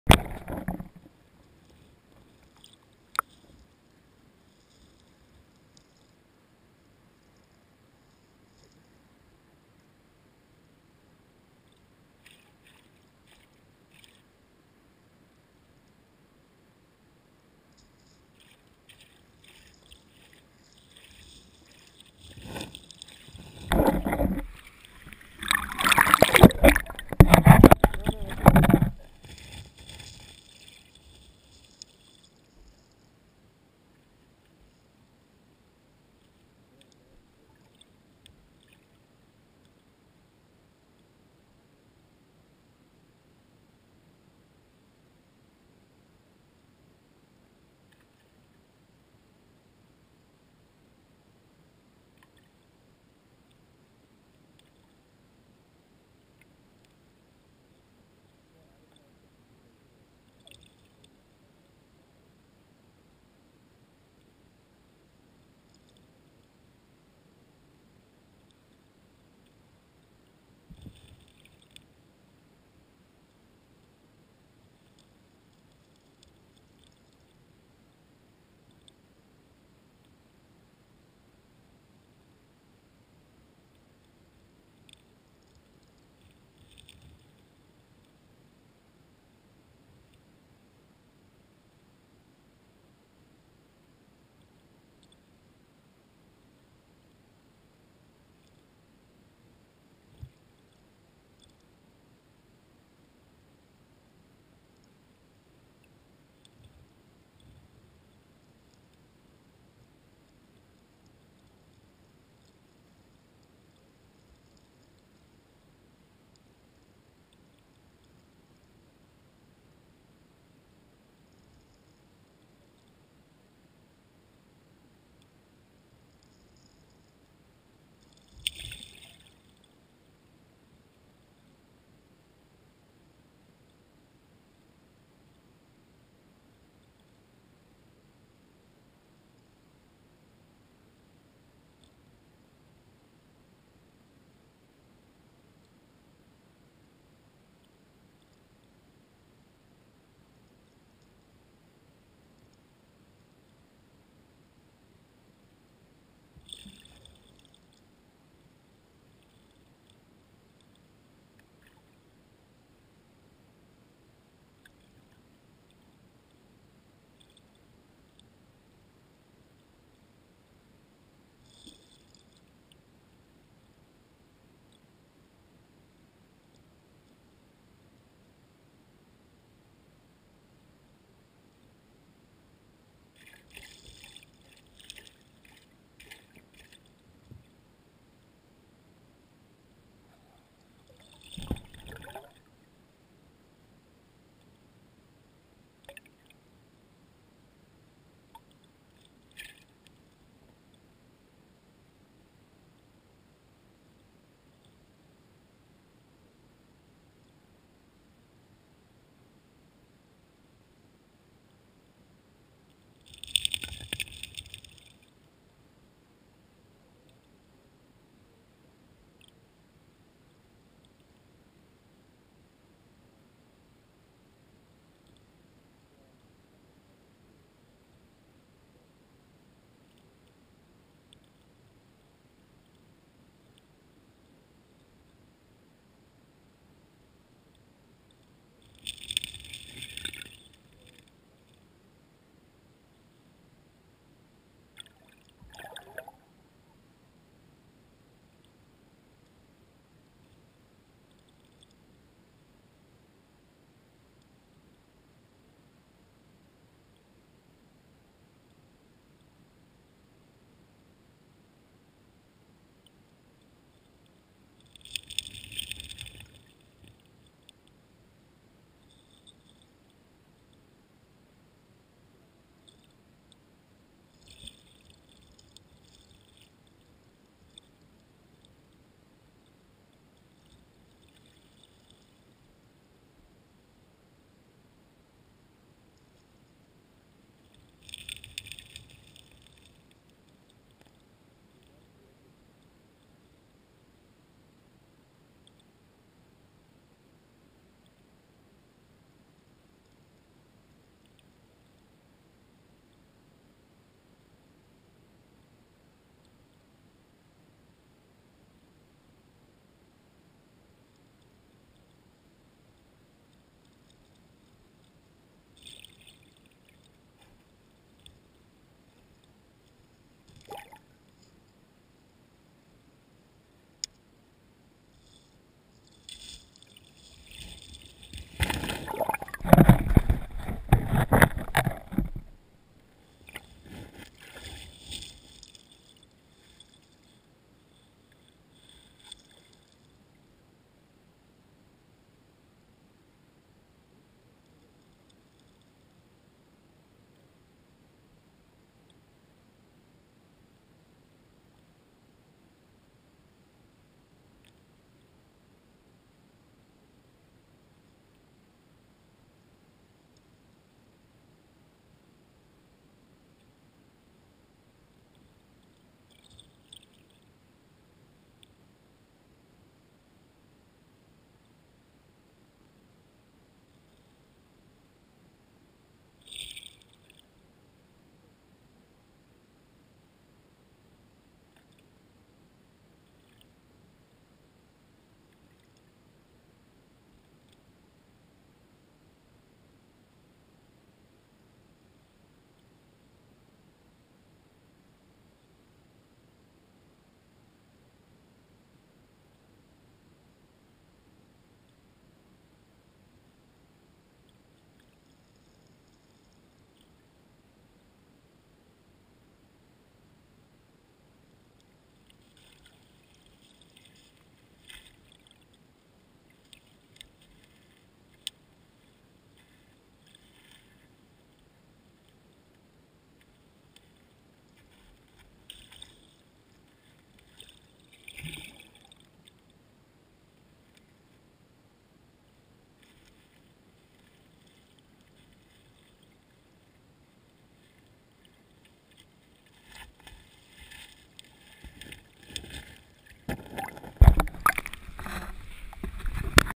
GoPro Underwater Salmon Spawn
Cascade river Coho Salmon party  filmed with GoPro Hero 2 960 48fps